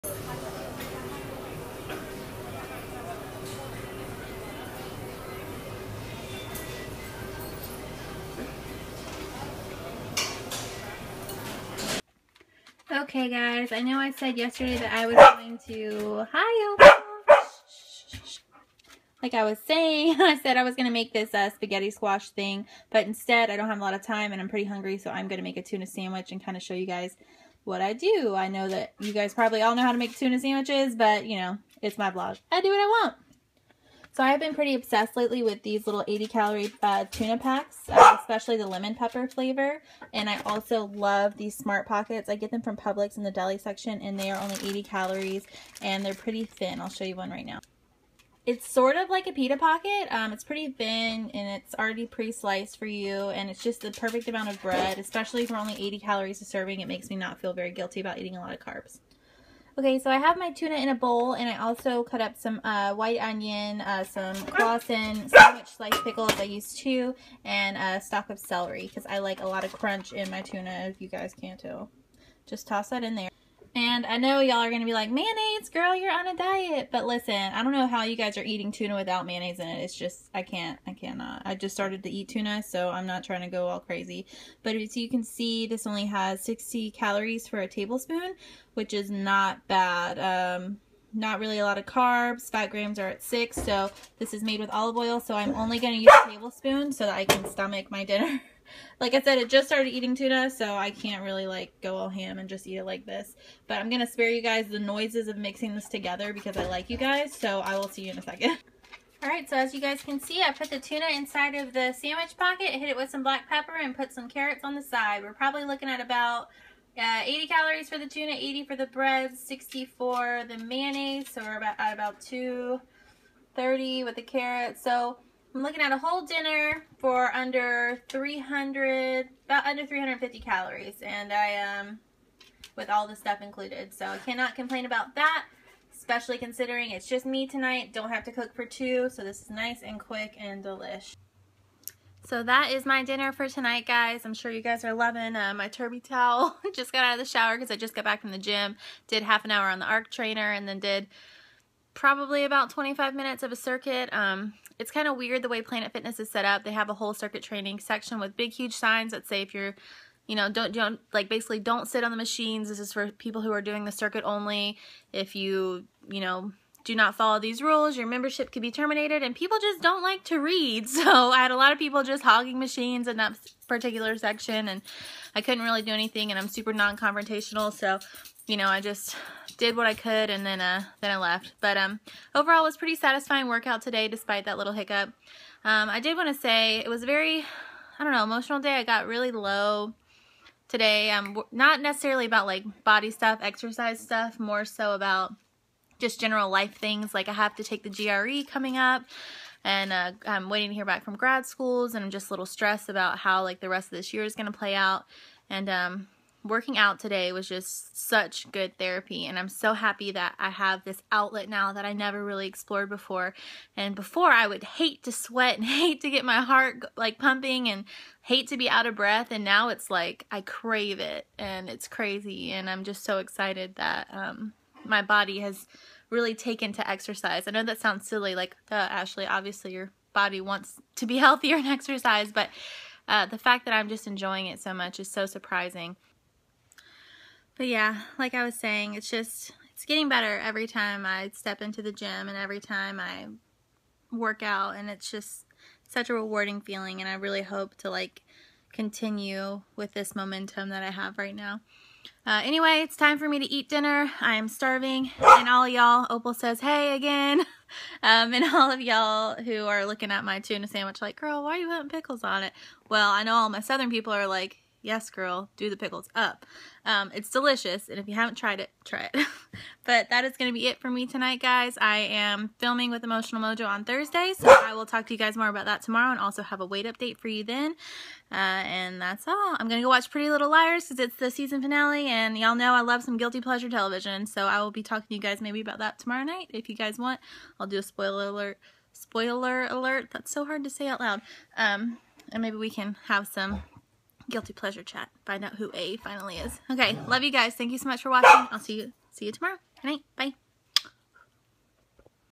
3/24/15 Day 60! 2 months down!!!
Day 60!!! Officially 2 months in! I show you guys my go to quick and easy dinner tonight! See you tomorrow! ❤️